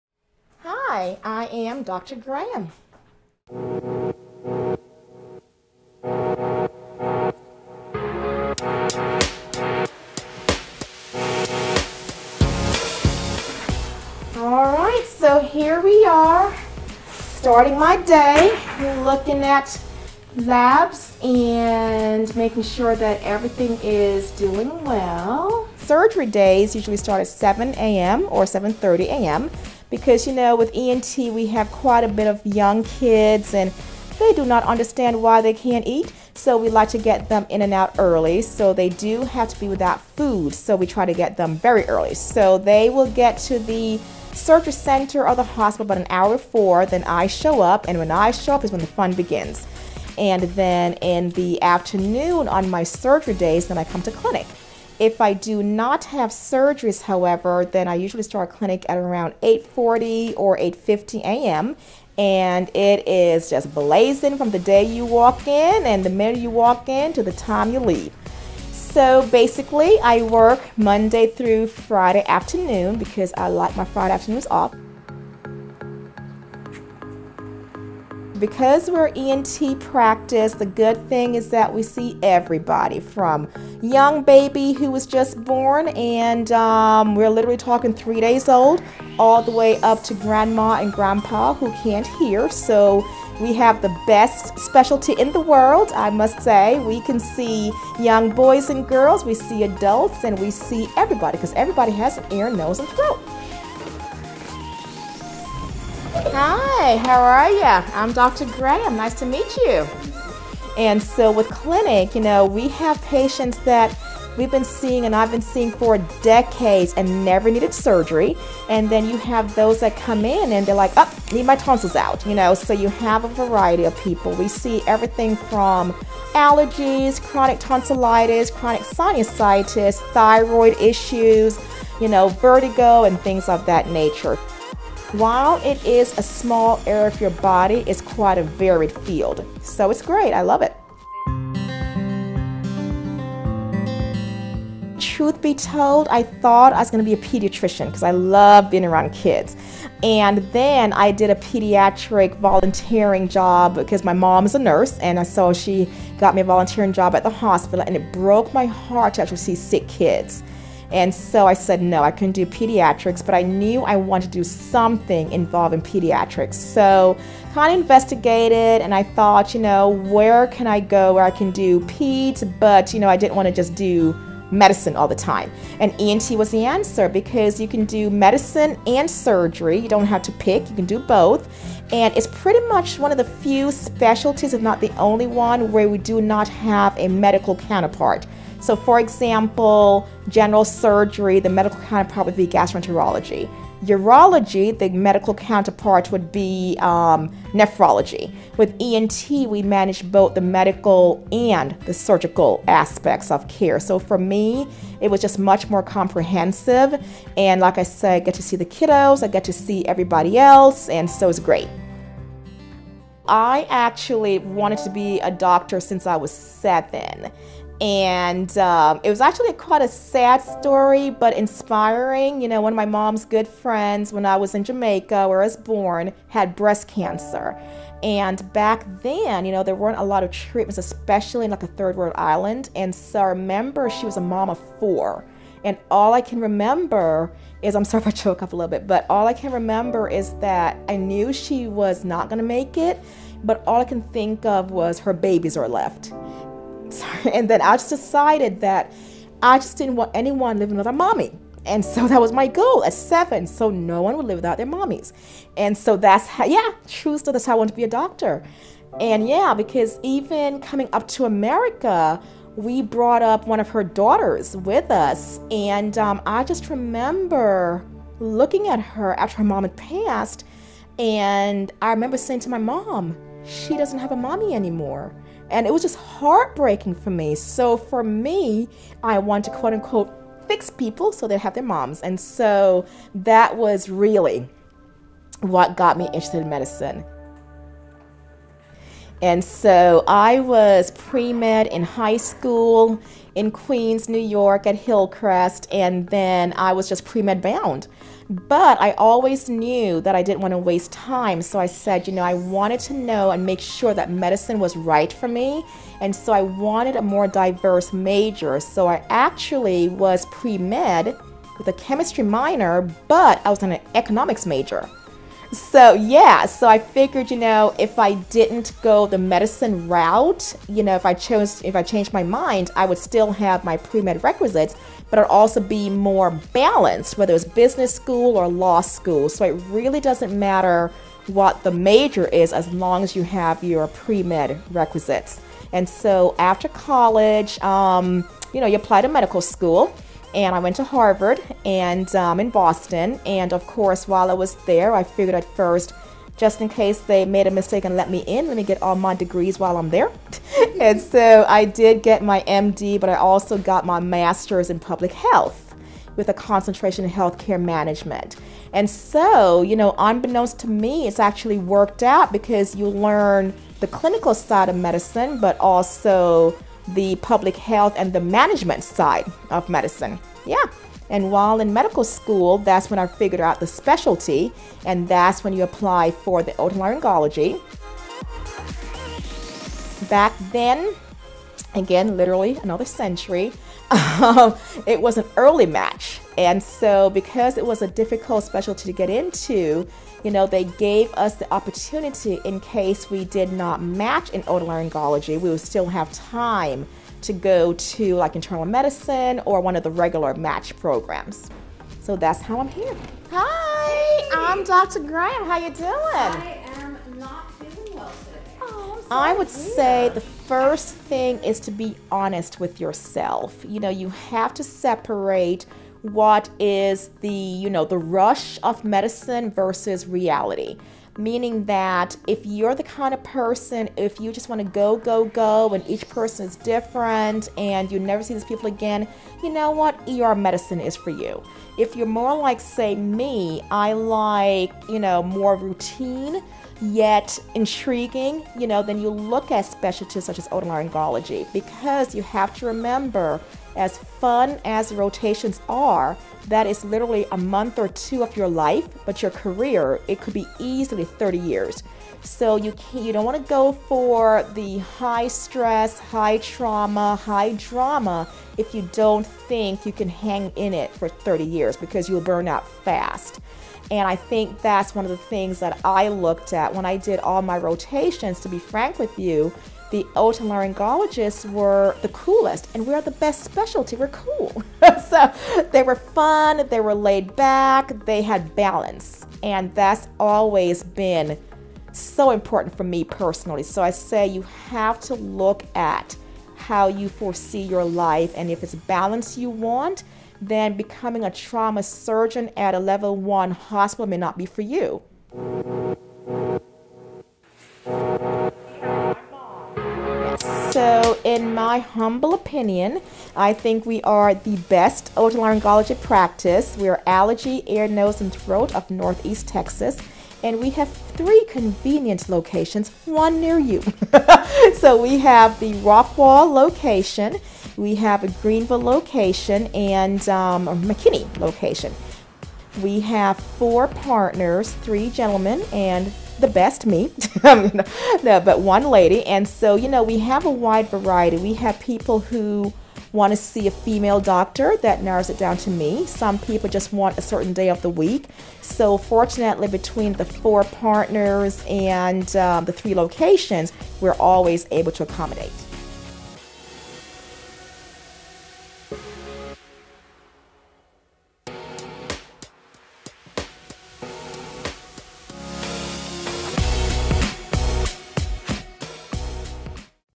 Day in the Life of an Otolaryngologist
In this video, you will see what its like to be an Otolaryngologist as Dr. Camille Graham shares with viewers what a typical day is, why she chose ENT, and advice for future students!

Timestamps:
0:00 Introduction
0:23 How she starts her day.
0:35 Why does her surgeries start early in the morning?
1:25 What type of patients does she see as an ENT surgeon?
2:22 Why she decided to enter the field of ENT?
3:35 When did she decide to become a doctor?
5:10 What was her major in college?
6:50 Advice for students when selecting a specialty
8:22 Why patients should choose her practice for ENT needs.

Who is Dr. Graham?
She is a Board Certified Otolaryngologist located in North Texas. She received her Bachelor Degree in Economics at Hunter College, New York, NY and graduated summa cum laude. Dr. Graham received her Medical Degree from Harvard Medical School and her Masters in Public Health Degree, also from Harvard. She completed her internship and residency at Henry Ford Hospital in Detroit, Michigan.

Her special interests are allergy, sinus disorders and pediatric ENT disorders.

Dr. Graham feels that the best way to practice medicine is when "technology is in harmony with nature". She is a health and wellness contributor to Scoreboard magazine, a publication that is currently found in luxury suits in sports arenas such as the American Airlines Center, Texas Rangers and Cowboys Stadium. She is also the host of a health and wellness radio program "On Call with Dr. Camille". Dr. Graham keeps up with the latest innovations in medicine and implements them with the herbs and supplements that Mother Nature provided us with, whether it is for chronic sinus disease, thyroid problems, allergies, ear infections or tonsillitis.

Dr. Graham utilizes state of the art equipment for her surgical procedures as well. These include balloon sinuplasty and image guided systems for sinus surgeries; coblation, bovie cautery for tonsillectomies and even one of the newest techniques for tonsillectomy, the ENTcep for tonsillectomies. She also performs minimally invasive head and neck surgeries including thyroid and parathyroid surgeries.